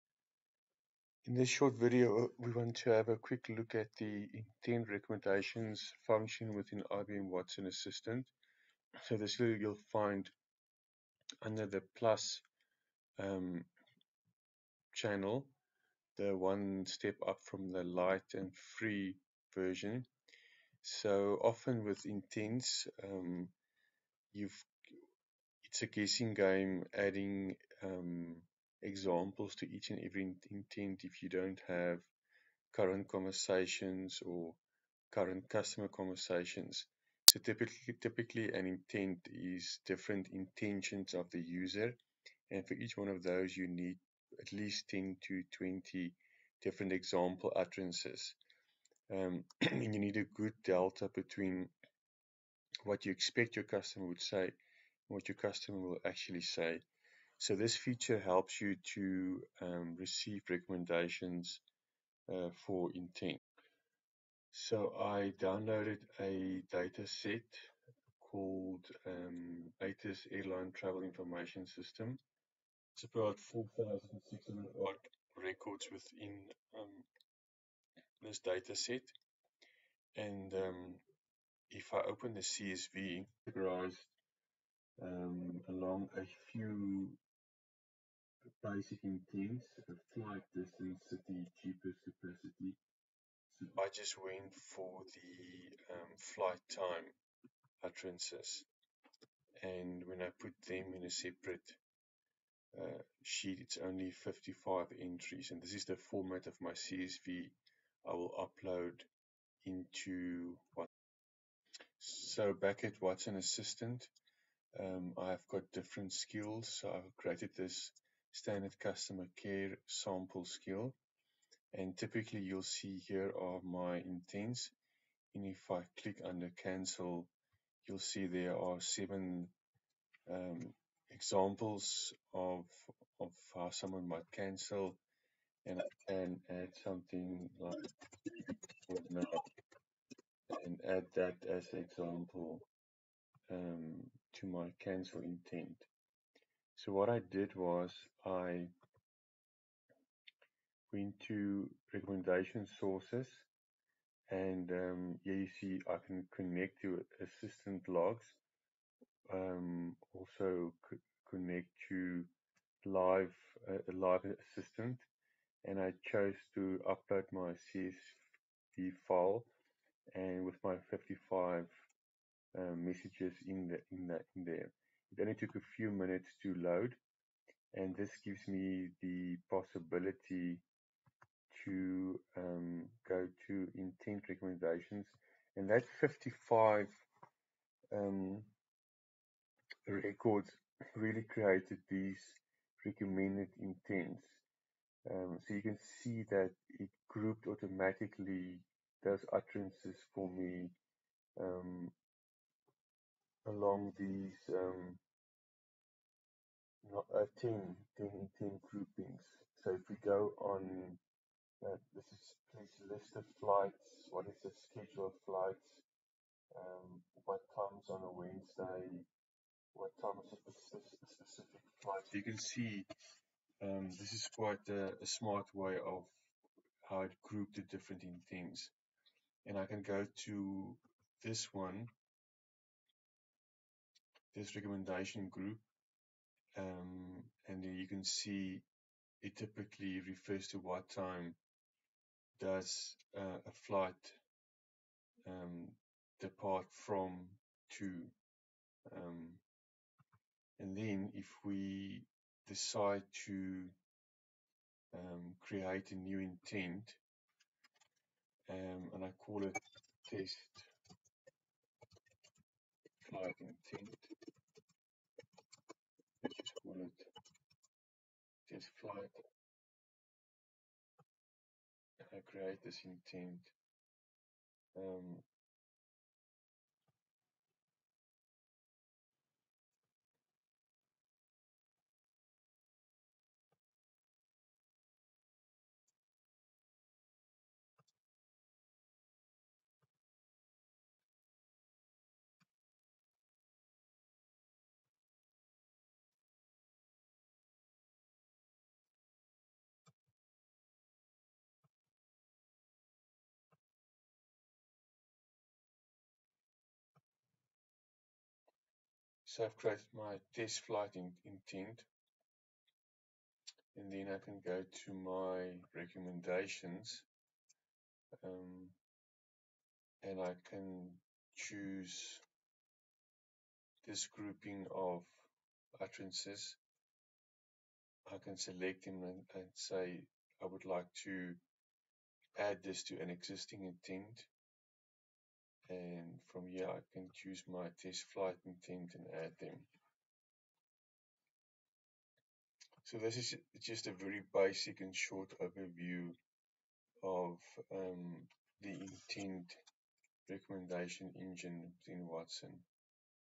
IBM Watson Assistant Intent Recommendations
Most often the first step in creating a chatbot is listing the different intents. Intents are really the different intentions a user might want to exercise in using your chatbot.

From this example Customer Care Sample Skill, the different intents are clearly care related to each other.

The first intent addressed, usually is the greeting, then the goodbye, followed by small talk.

The key is to segment the intents accurately, and not have conflicts. And not have too many or too little intents.

If there are too many intents, conflicts are inevitable, with duplication of data. If there are too little, the chatbot will invariably not address the query and need of the user. You will start seeing scenarios like fallback proliferation.

This is the challenge, having the intents granular, but not too granular. And also the inverse is true.

The test environment is IBM Watson Assistant Plus. Once we have uploaded the the data, Watson evaluates the user utterances and identifies common problem areas that customers mention frequently.

Watson Assistant then displays a set of discrete groups of related candidate user examples that capture the trending customer needs. The groups are so granular in scope that you might choose to add separately-grouped candidate user examples to the same intent.

You can review each recommended intent and the corresponding user examples to choose the ones you want to add to your training data.